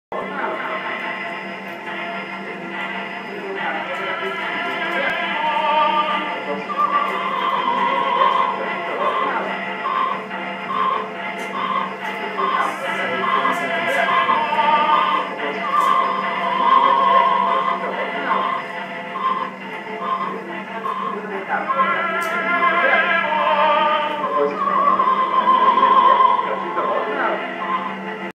Tapebox and his analogue loop setup at Shinytoys Space 2012
Dan Hayhurst (tapebox/Sculpture) shows his analogue loop setup at Shinytoys Space 2012.